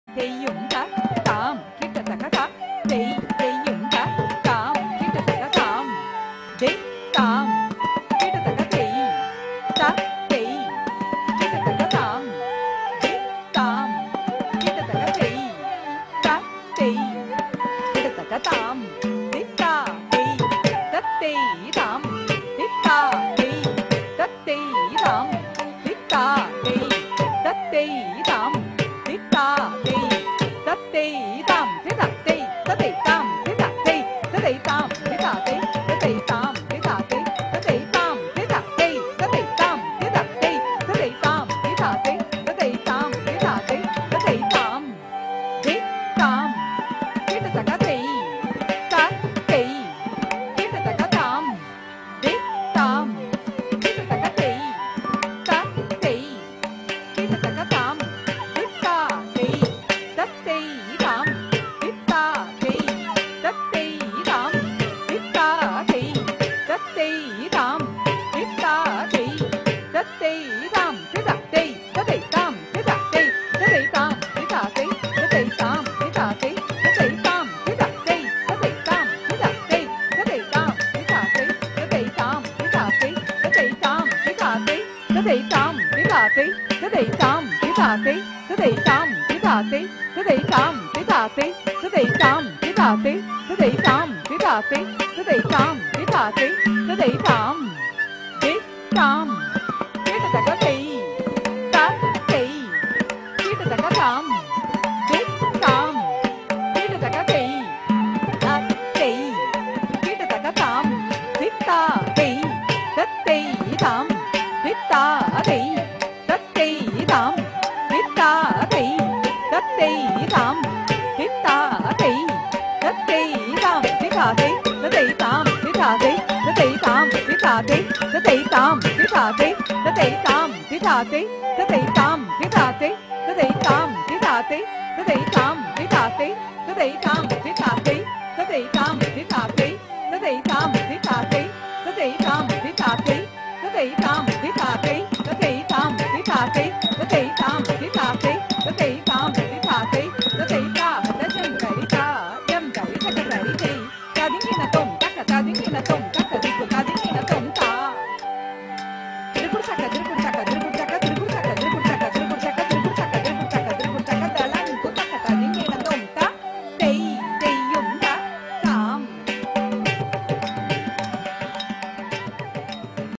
Pratibha Parayanam Mahotsav (International Platform for Art & Culture)
Hriddhi Protim Sil performed Bharatnatyam in Senior category.